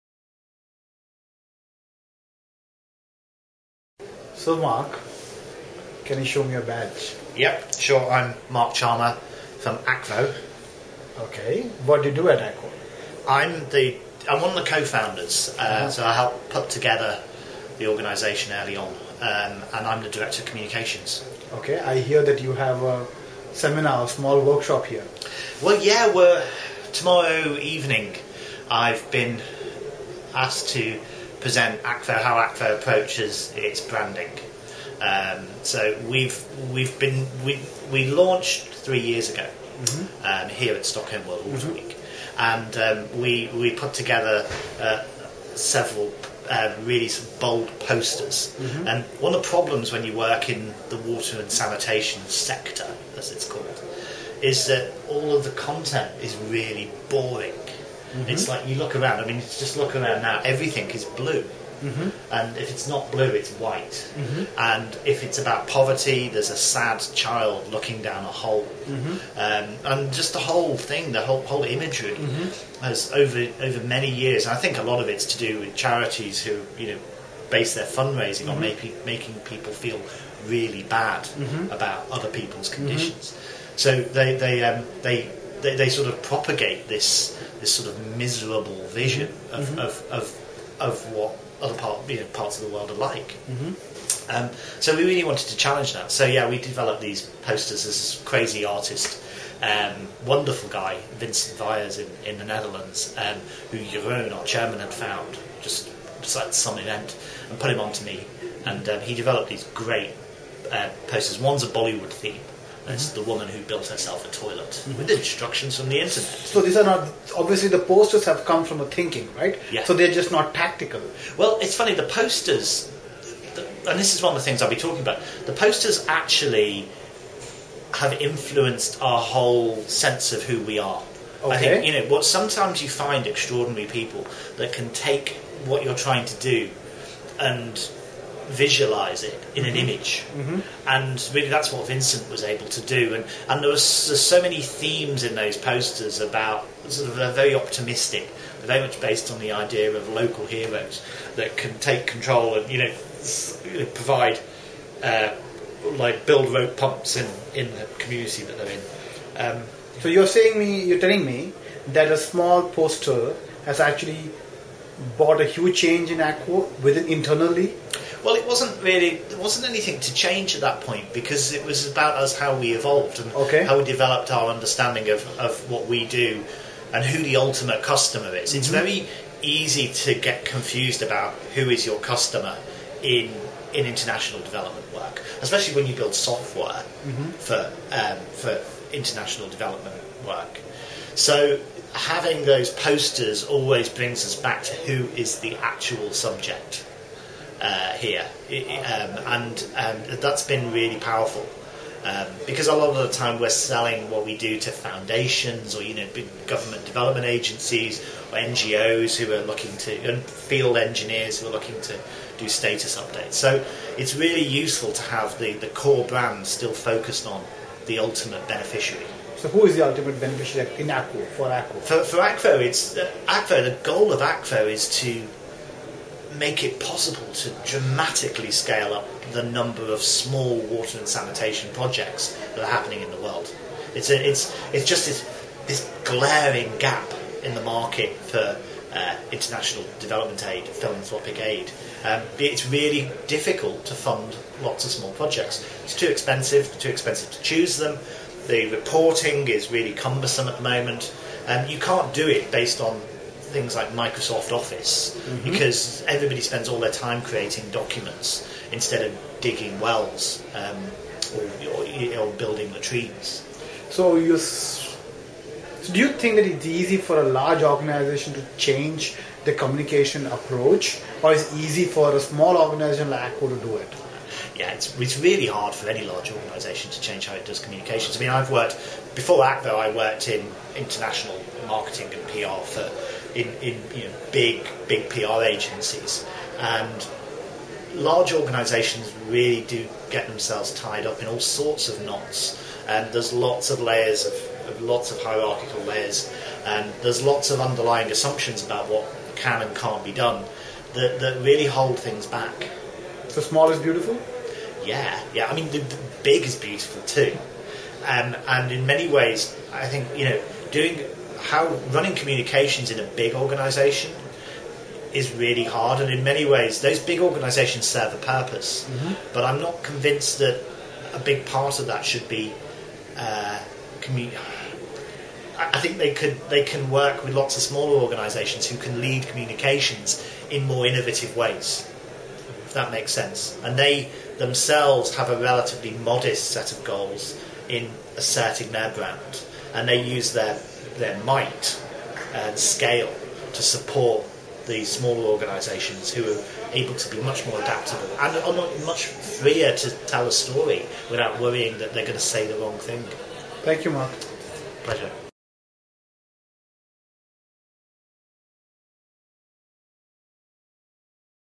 Mark Charmer from Akvo on modern nonprofit communications
Mark Charmer from Akvo on modern nonprofit communications (Sep  8, 2010)